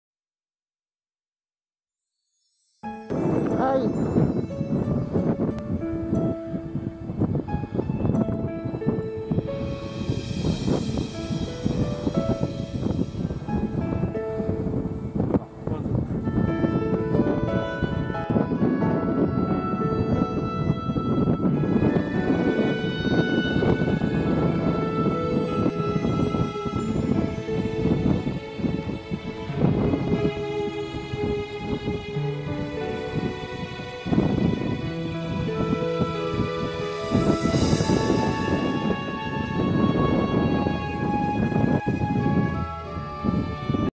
2021/2/3 Kさん Launchmodel DG-600 3.6m 着陸
綺麗に着陸します。